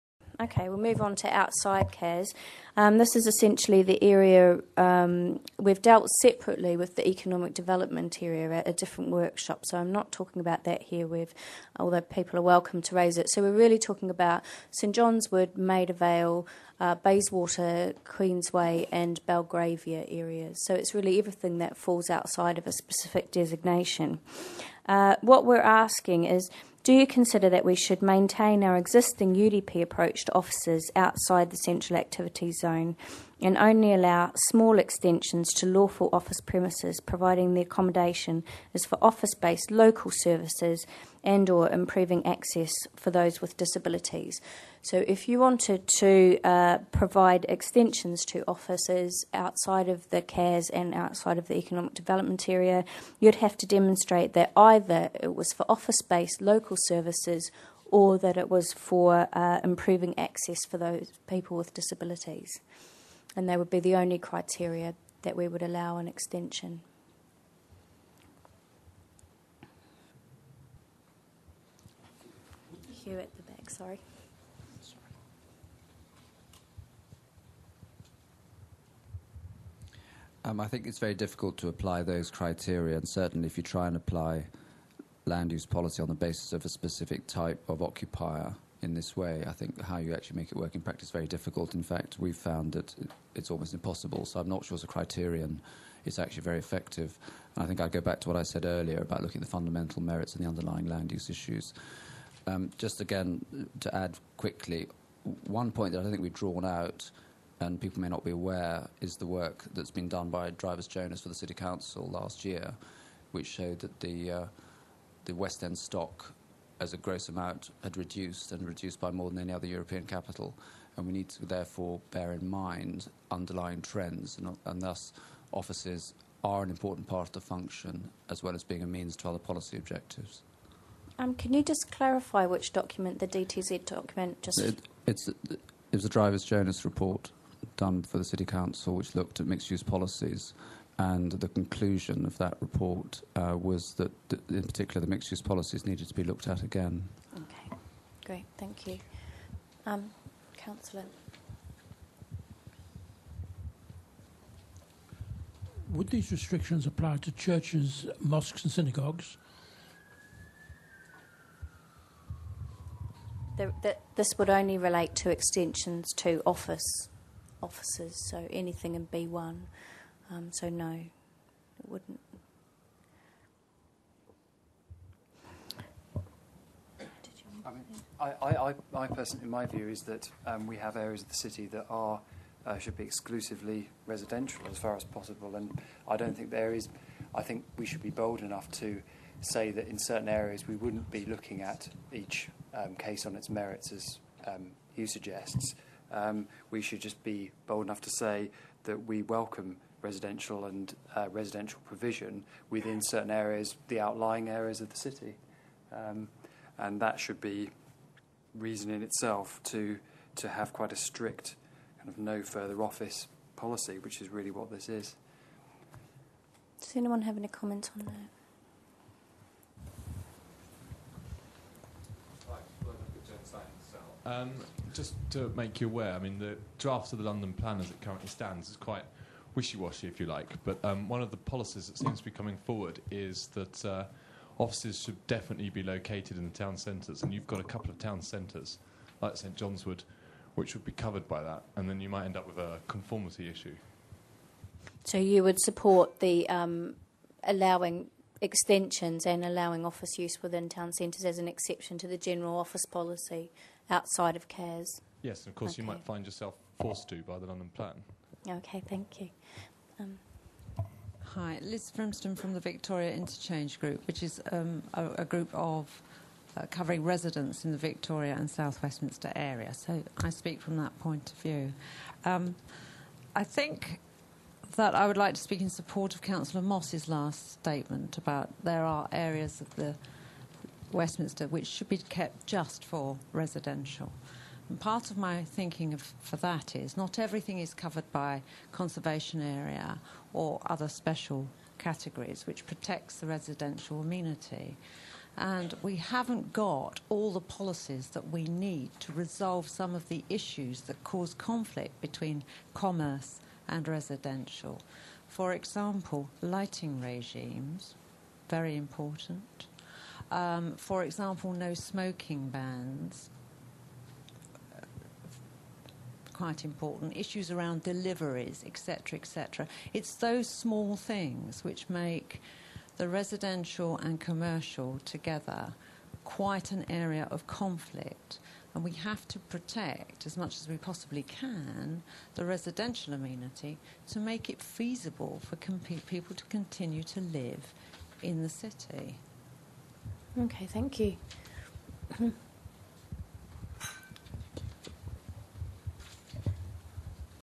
Commercial Activity Part 1A Q5
These are the recordings for the City Management Plan Workshop which took place on evenings in June, July and August 2009.

Commercial Activity Part 1A Q5: 5. Do you consider that we should maintain our existing UDP approach to offices outside the Central Activities Zone and only allow small extensions to lawful office premises providing the accommodation is for office based local services and/or improving access for those with disabilities.

Please note these are recorded public workshops therefore the answers to the questions do not always follow the question. Some issues raised for discussion are dealt with in more than one workshop. Therefore to hear the full discussion you may wish to listen to the whole workshop recording and/or related topic areas.